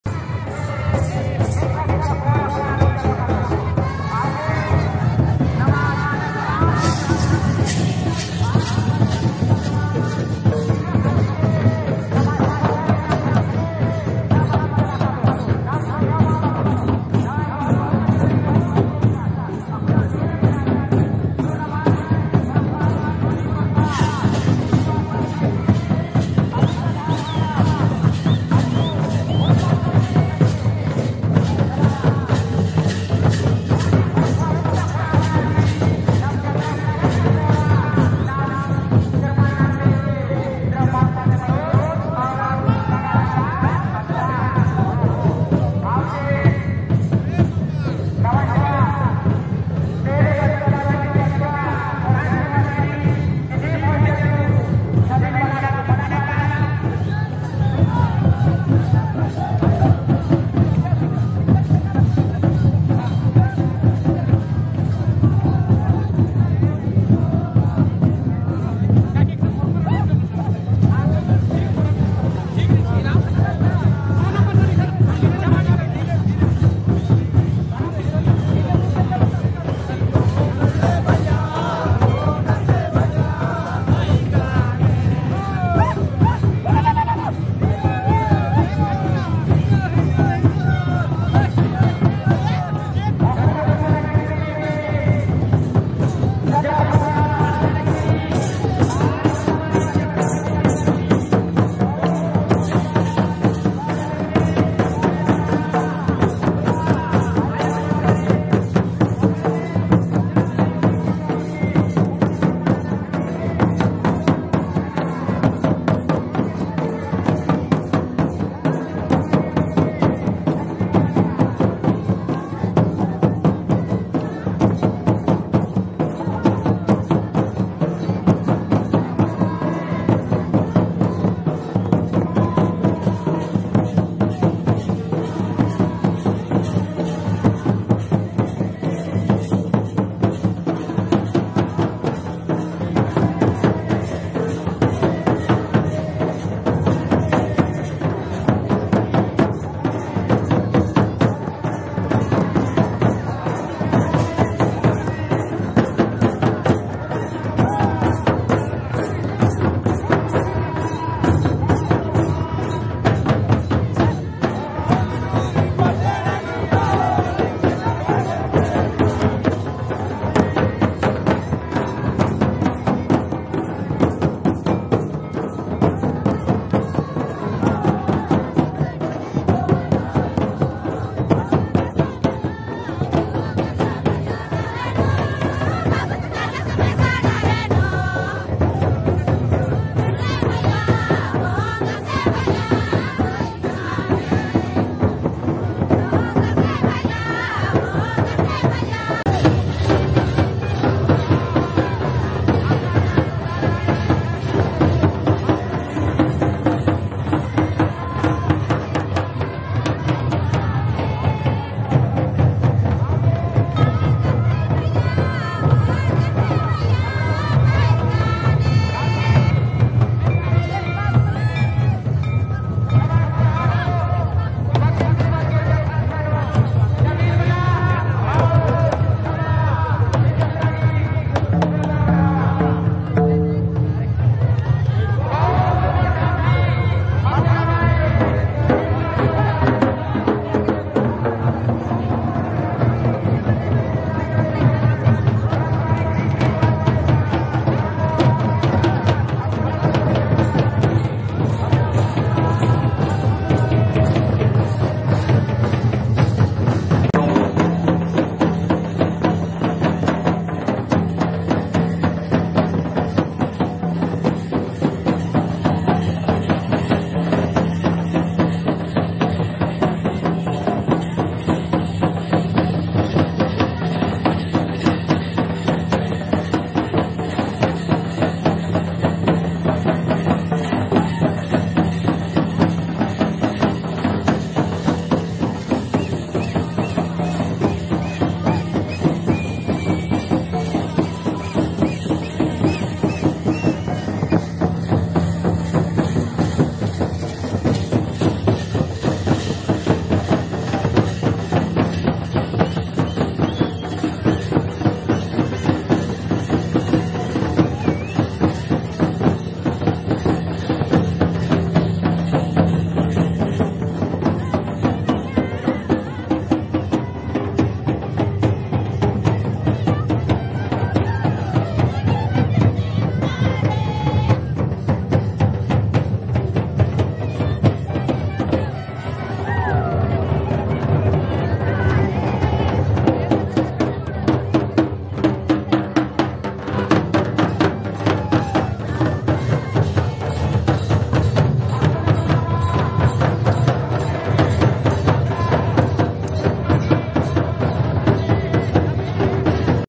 सात पाड़हा झंझरी जतरा!!47 खोड़हा का आगमन एक बार जरूर देखे आदिवासियों का रीति रिवाज एवं परंपरा🙏🙏
सत पाड़हा झंझरी जतरा